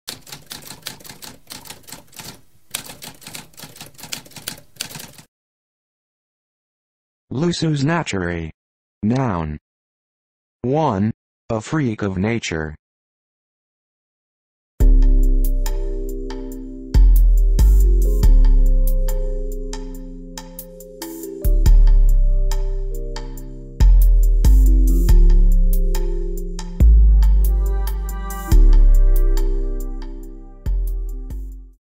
What does lusus naturae mean?
A spoken definition of lusus naturae.

Intro Sound:
Typewriter - Tamskp